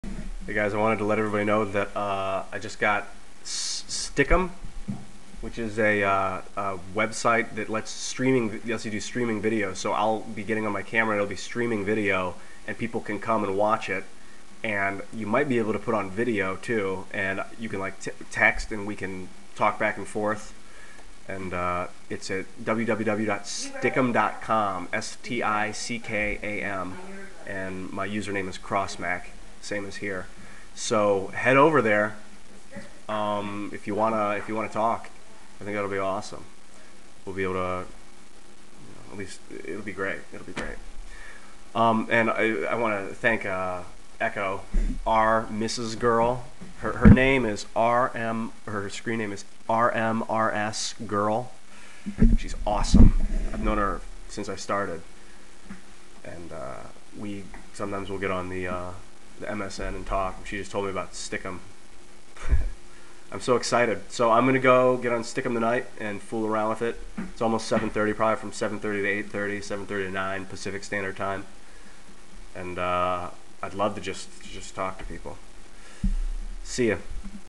I just got STICKAM
I'm going to be on Stickam tonight for a while.  I'm going to start using it sometimes, it's great.

my username: Crossmack

come chat